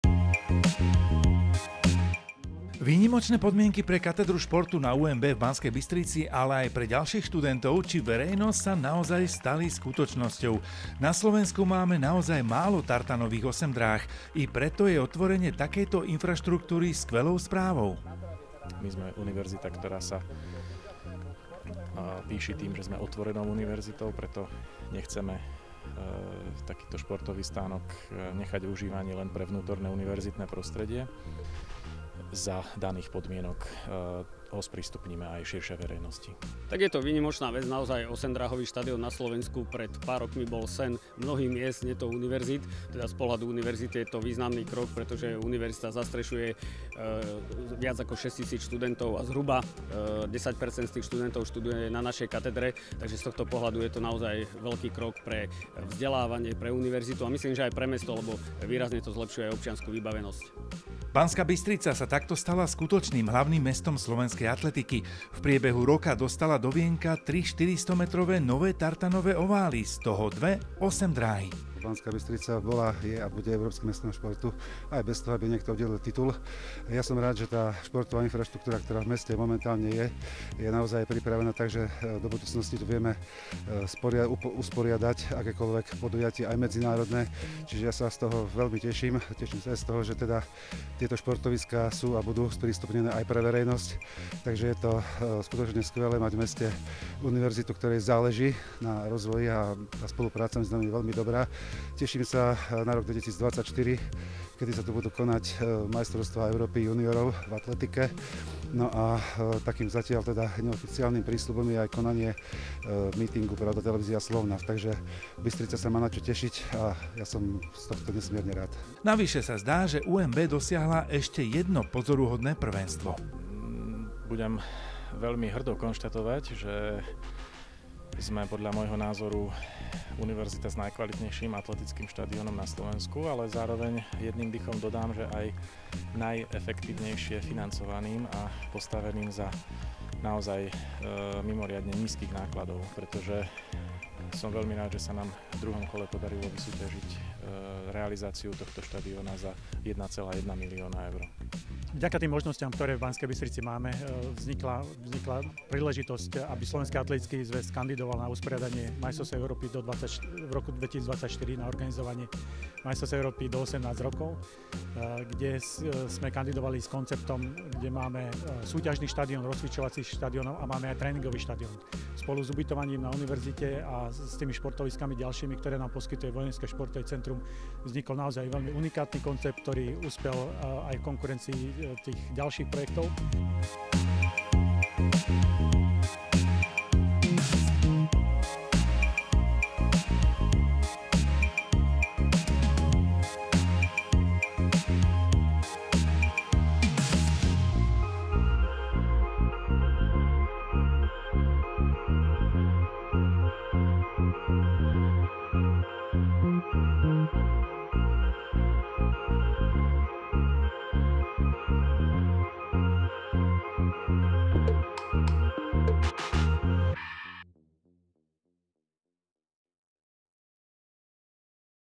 štadión UMB otvorenie, 25.10.2022
Na výstavbe atletickej infraštruktúry najviac získala Banská Bystrica. Okrem investície do veľkého štadióna na Štiavničkách sa už môže aj Univerzita Mateja Bela pýšiť nádhernou osemdráhou